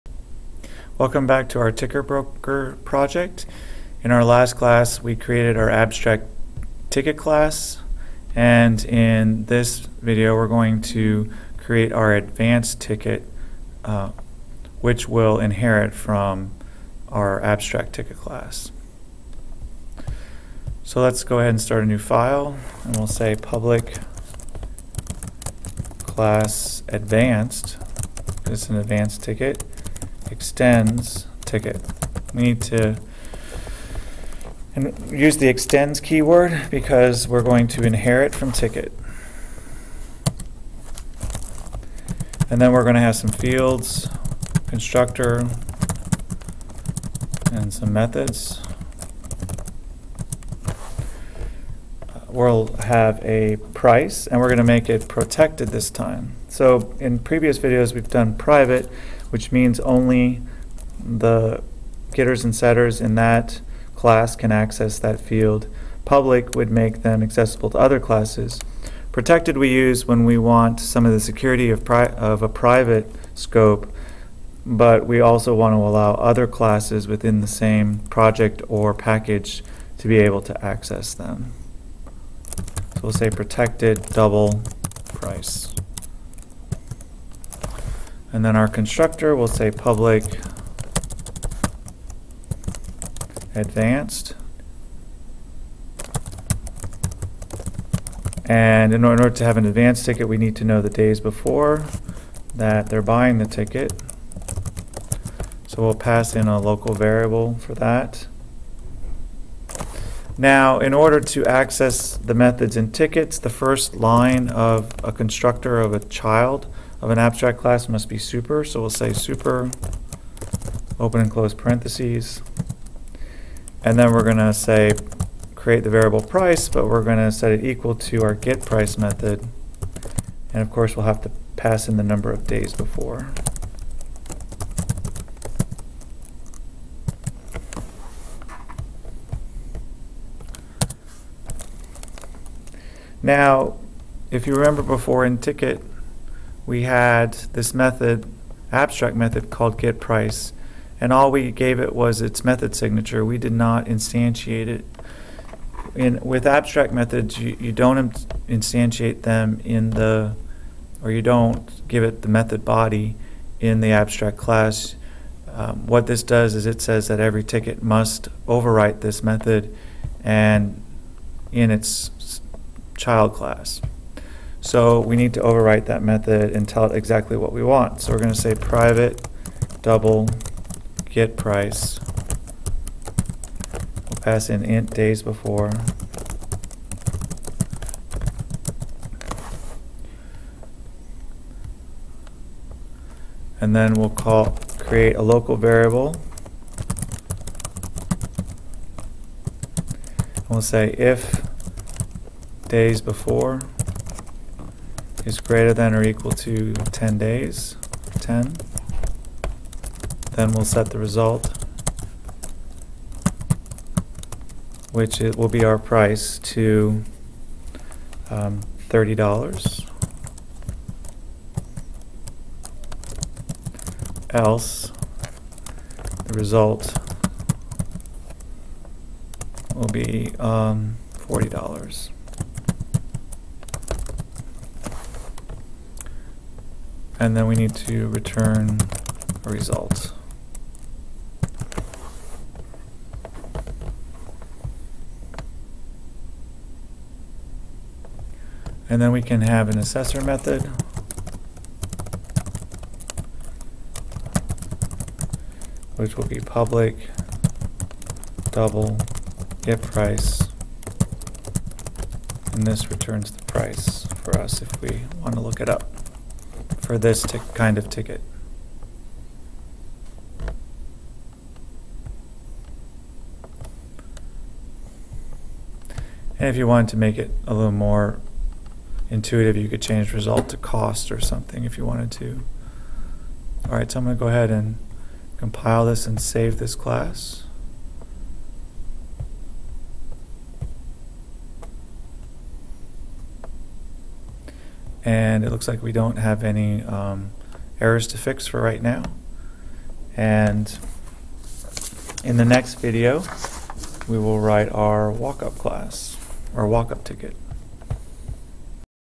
Ticket Broker Part 3 Advanced Ticket
This ticket is for someone who buys there tickets in advance.